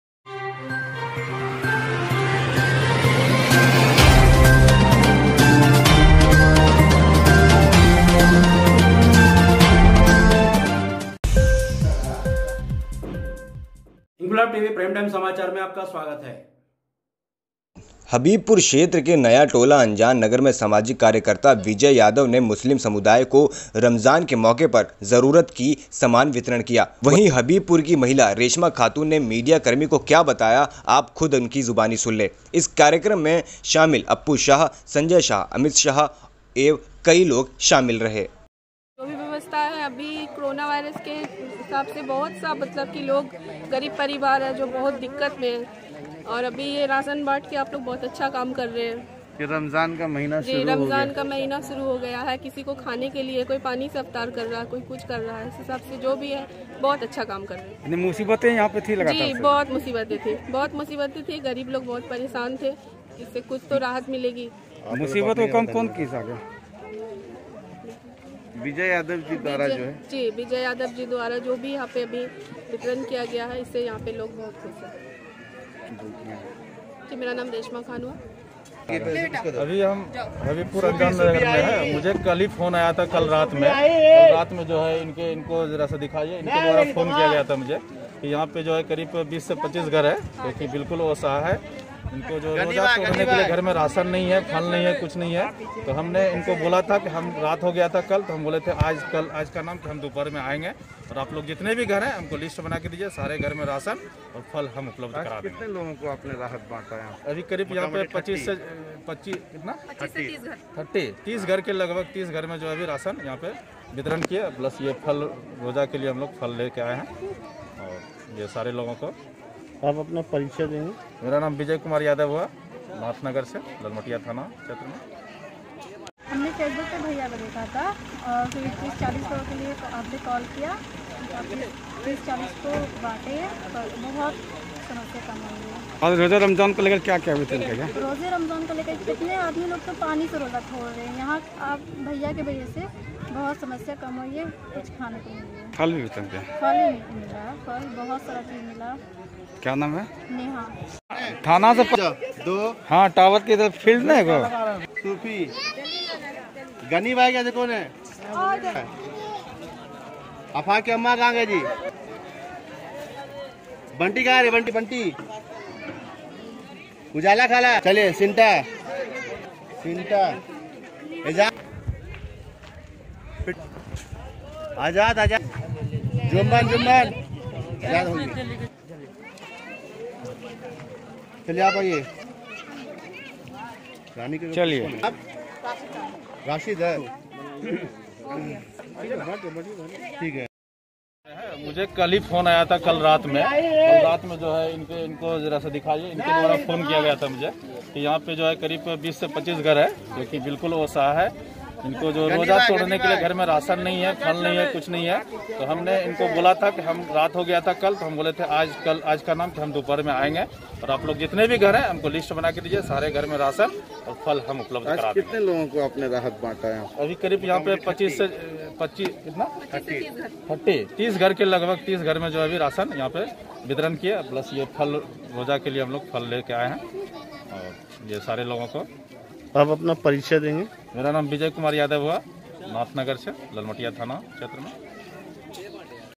INQUILAB TV#सामाजिक कार्यकर्ता विजय यादव ने जरूरत का सामान वितरण किया
भागलपुर हबीबपुर मैं सामाजिक कार्यकर्ता विजय यादव ने जरूरत का सामान वितरण किया

हबीबपुर क्षेत्र के नया टोला अंजान नगर में सामाजिक कार्यकर्ता विजय यादव ने मुस्लिम समुदाय को रमजान के मौके पर जरूरत की सामान वितरण किया वही हबीबपुर की महिला रेशमा  खातून ने मीडिया कर्मी को क्या बताया  आप खुद  उनकी जवानी सुन ले ! इस कार्यक्रम में शामिल अप्पू शाह. संजय शाह. अमित शाह. एवं कई लोग शामिल थे